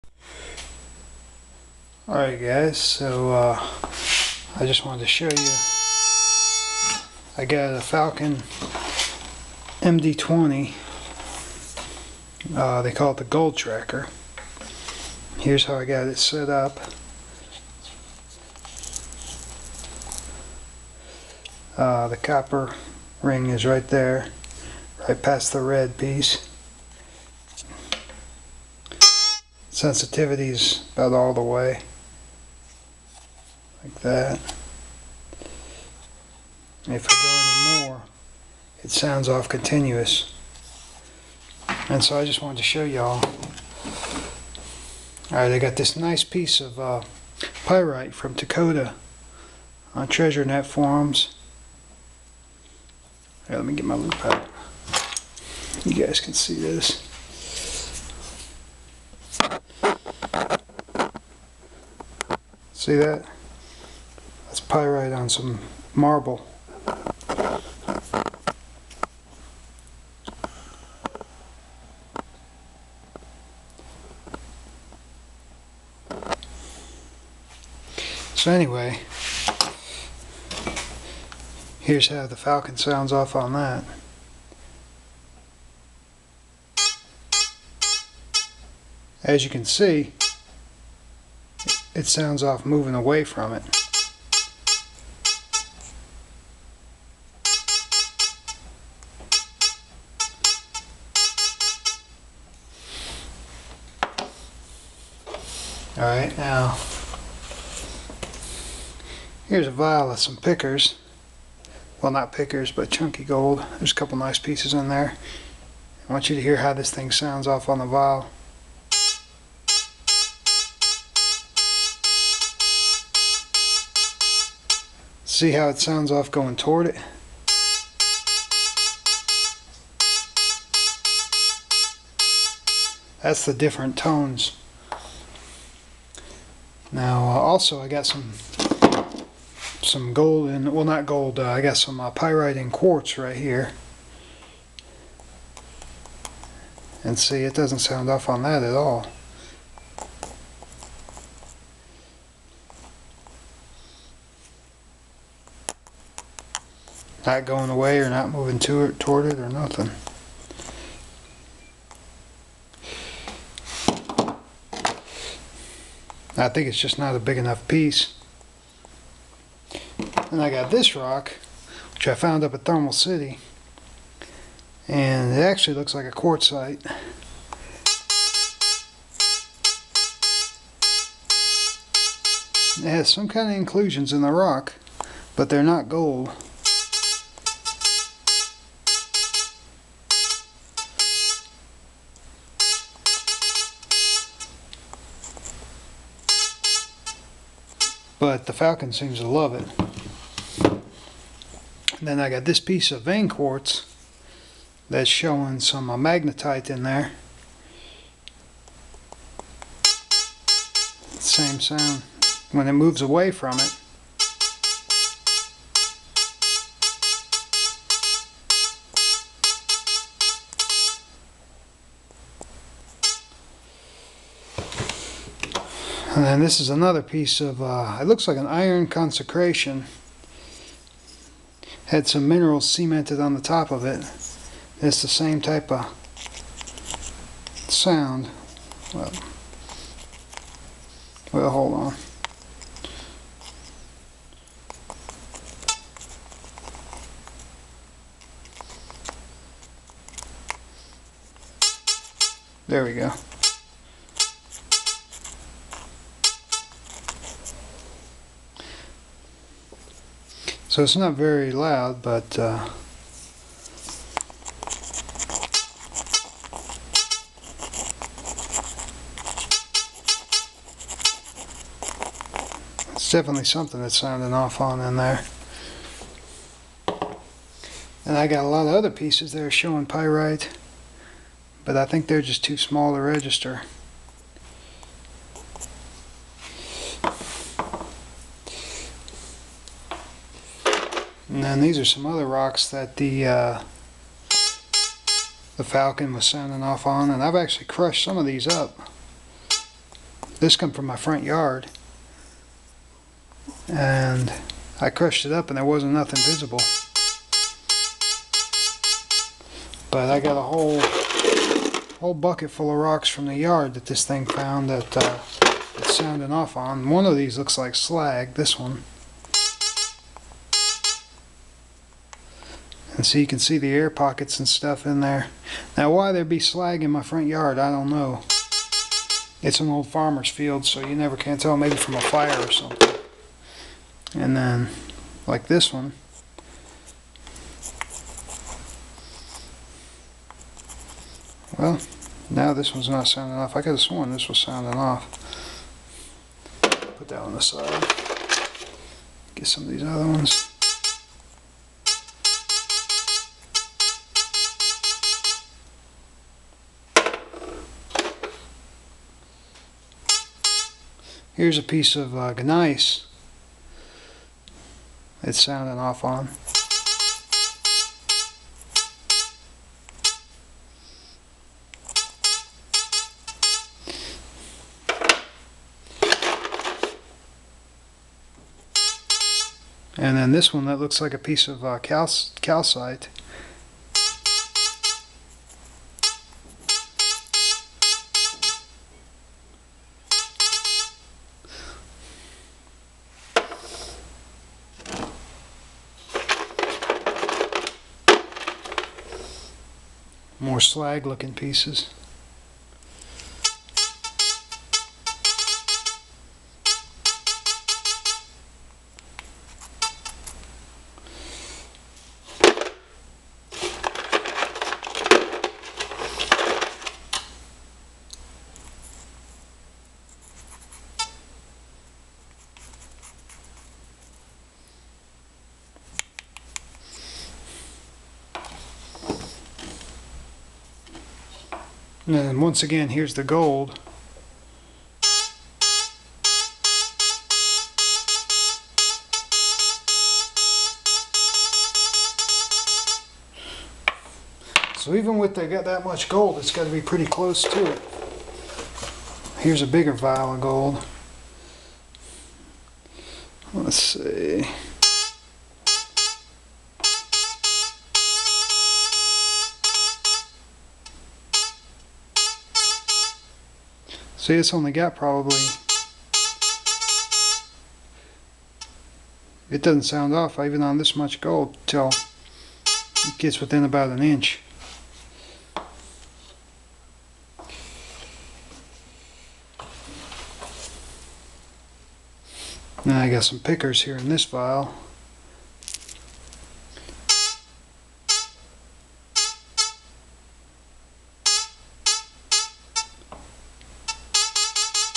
Using a Falcon MD 20 Gold Tracker (Part 1)
Watch in 720pHD for best picture! A video to show how my falcon md20 gold tracker sounds off on some pyrite and also some gold.If there's enough interest I will post a better video showing how small of gold this thing will sound off on.